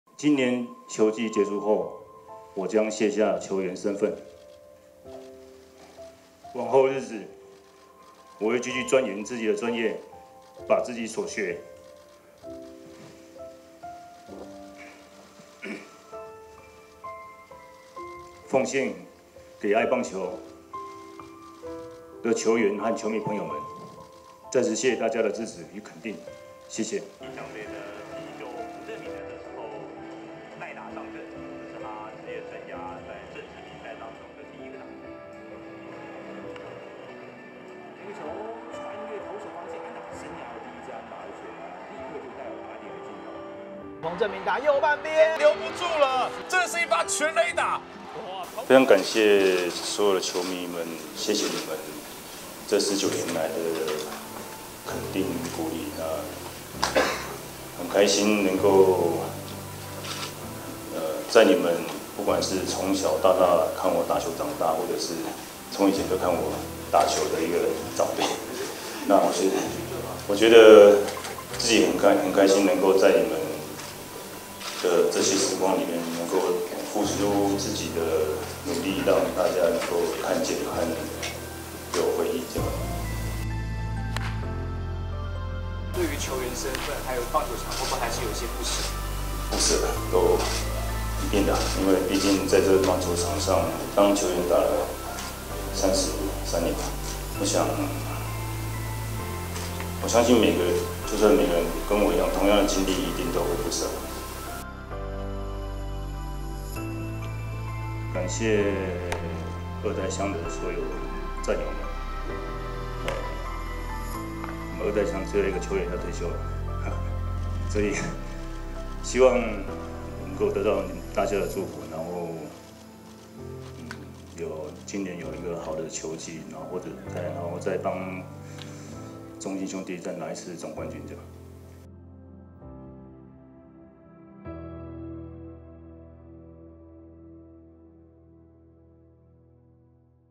中職／最後一位二代象 恰恰彭政閔哽咽宣布退休
19年來帶給球迷們無數回憶
而今年的9月29日，就是恰恰的引退日